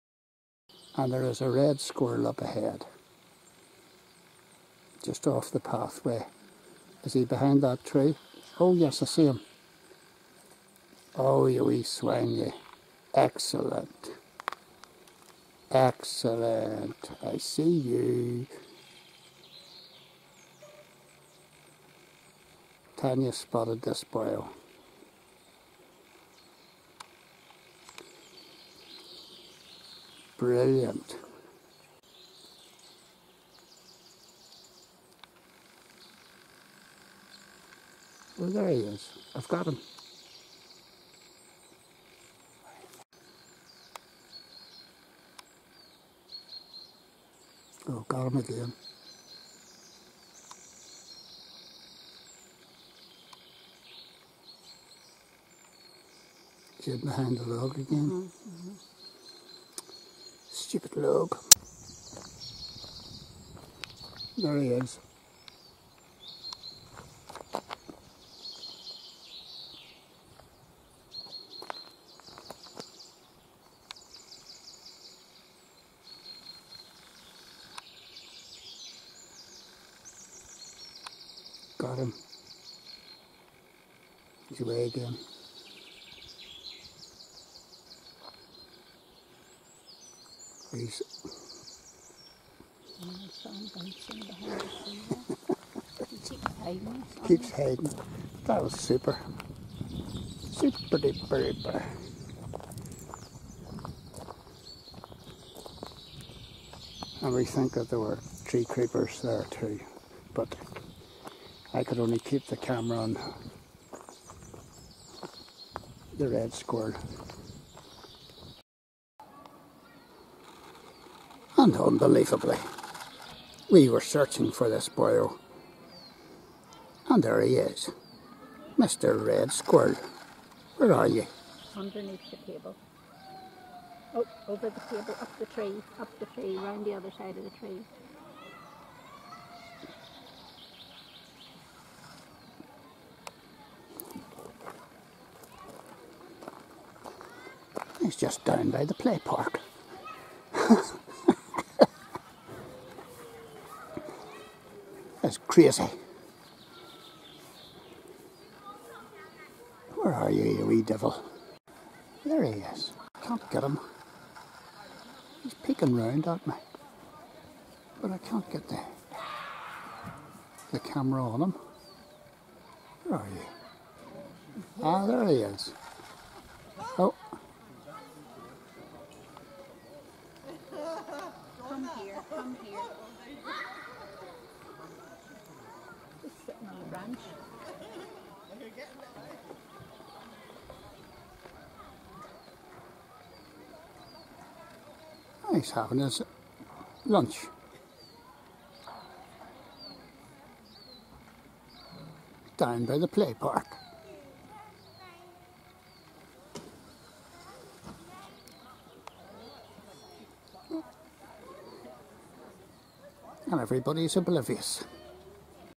Red Squirrel Frustration Gortin Glen Omagh -Shaky
Not the best footage I've ever taken but hey, I managed to capture this frustratingly elusive red squirrel on video and I'm well pleased. I've never filmed one before. This was on the last day of our short mini break to Omagh when we were having a dander through Gortin Glen.

We haven't seen a red squirrel for decades. Belvoir forest used to have 5 or 6 then some 'smart' person moved the feeder from the main carpark and they disappeared. The fact that the grey squirrel populations have been allowed to get comletely out of control everywhere doesn't help the red cause.  Maybe the increase in pine martin numbers will sort out some of the grey problem and allow the reds to recover?